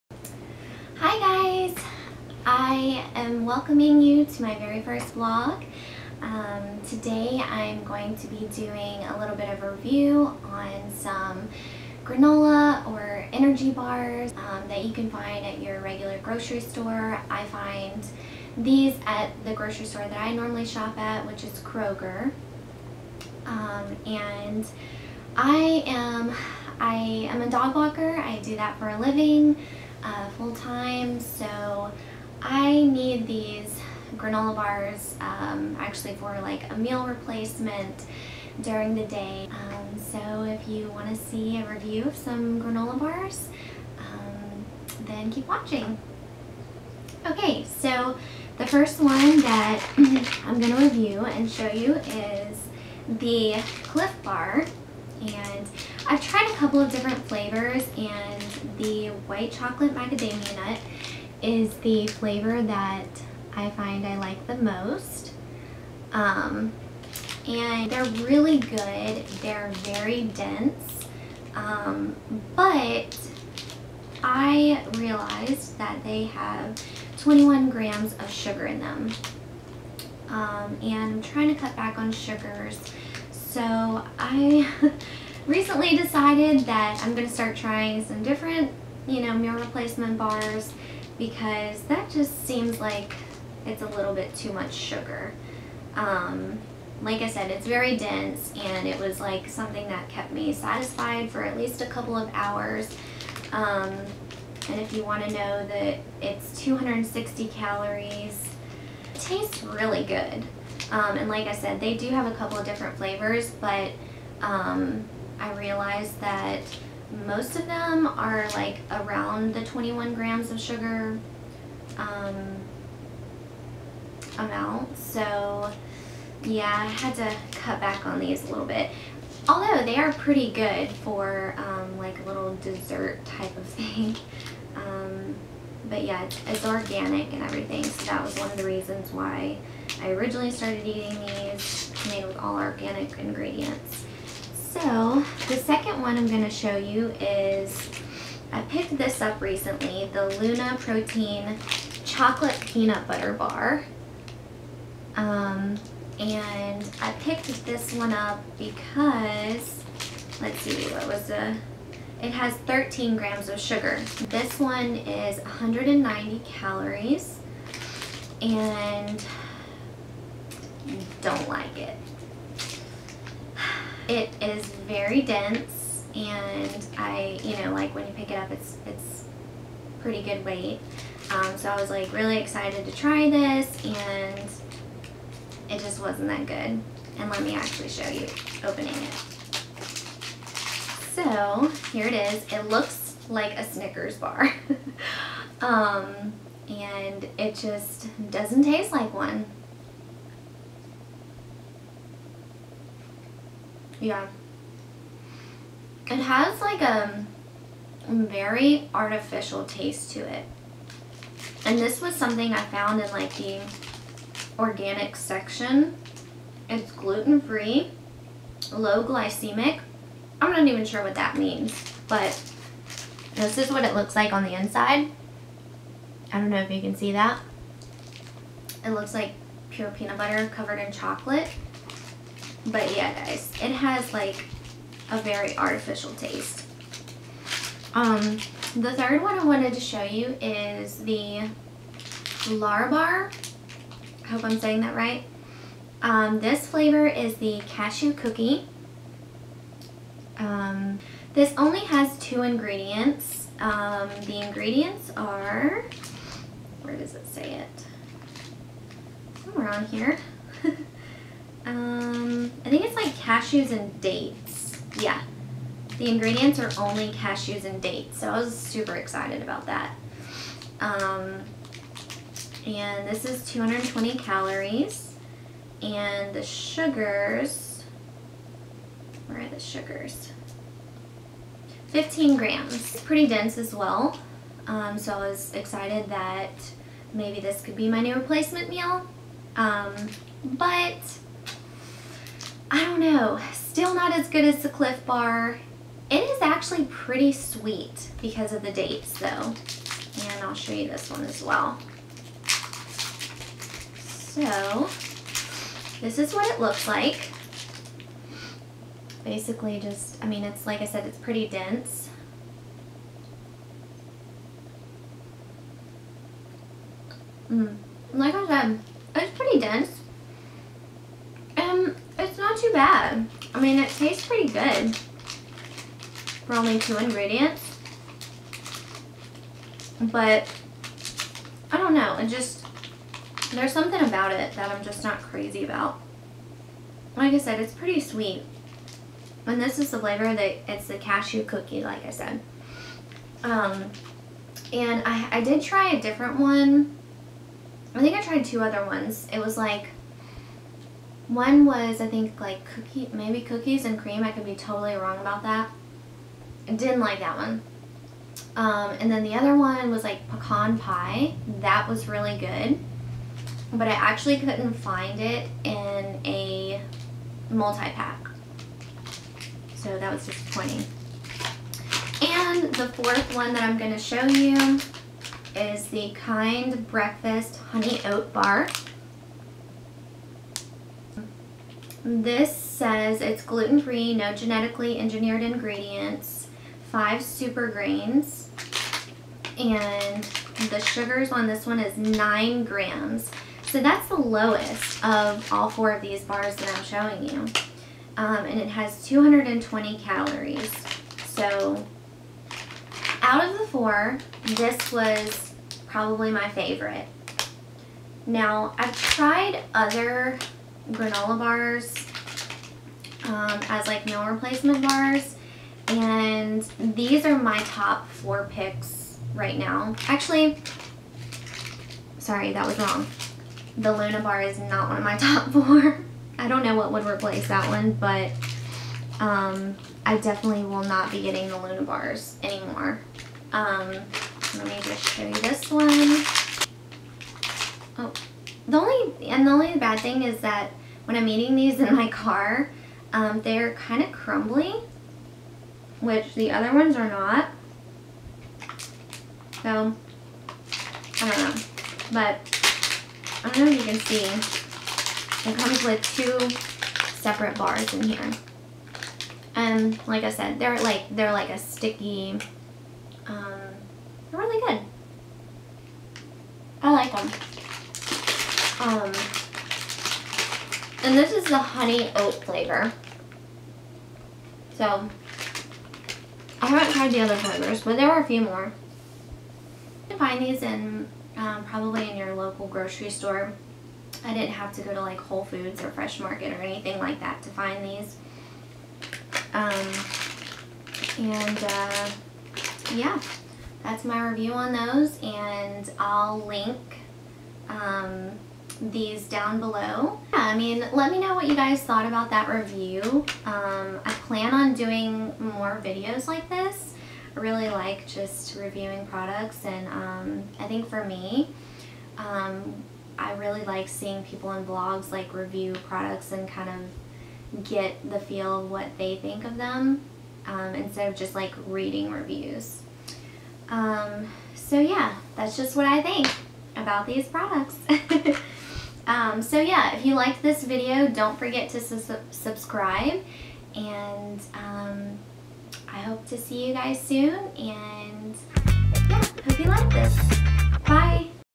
ENERGY/GRANOLA BAR | TASTE TEST
In this video I taste test four (granola/energy/protein) bars and give you a short review!

I plan to put out more videos like this as well as some others! I'm so excited to finally join the YouTube family and really hope you guys enjoy!

Much Love!

SHOP THIS VLOG:
CLIF BAR
KIND BAR
LUNA BAR

MY CAMERA